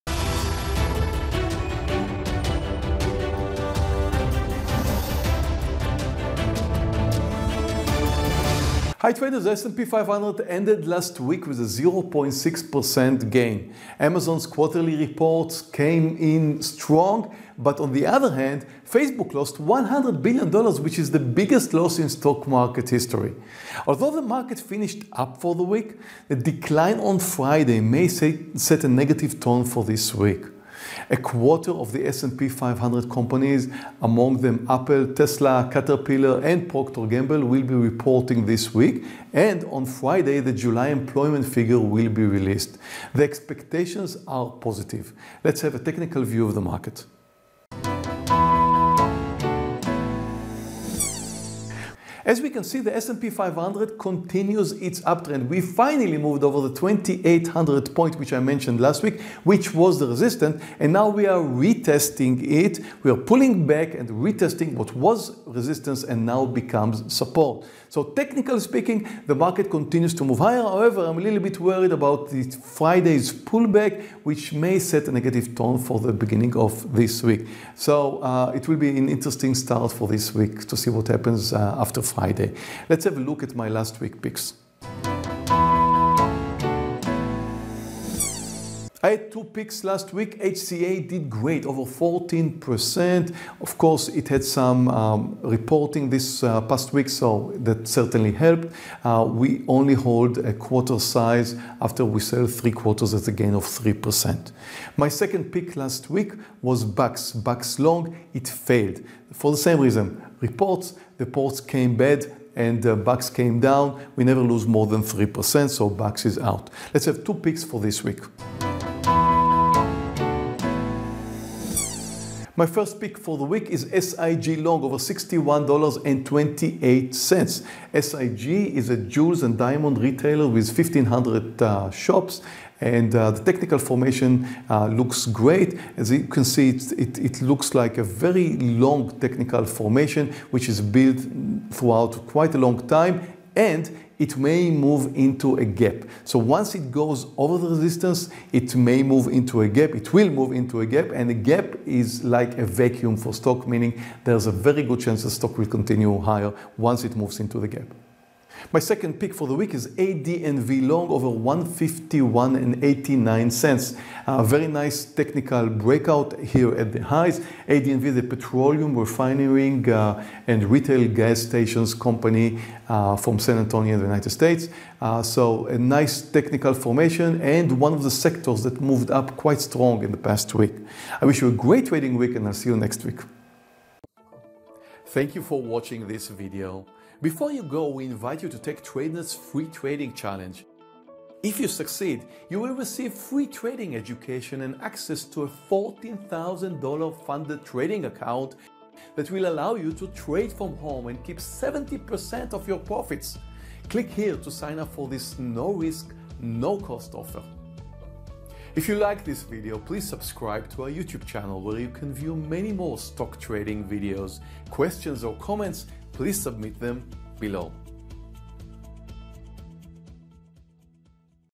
Swing Trading Weekly Review - July 29th.
Swing Trading Weekly Review - July 29th.: The S&P 500 ended last week with a 0.6% gain. Amazon's quarterly earnings reports came strong, but on the other hand, Facebook lost $100 billion- the biggest loss in stock market history. Although the market finished up for the week, The decline on Friday, may set a negative start for this week. A quarter of The S&P 500 companies, among them: AAPL, TSLA, CAT and PG will be reporting this week, and on Friday the July employment figure will be released. The expectations are positive .Let’s have a technical view of the market.